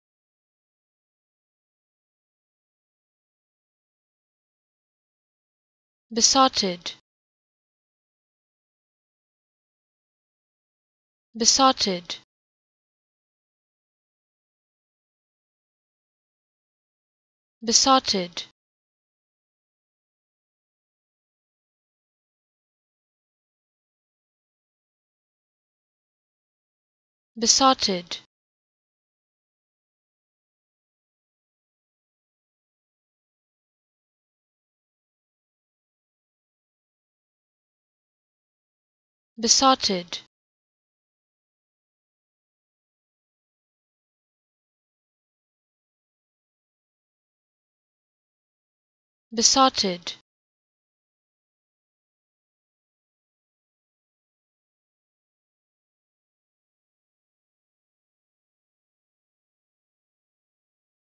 How To Say Besotted
Learn how to say Besotted with EmmaSaying free pronunciation tutorials.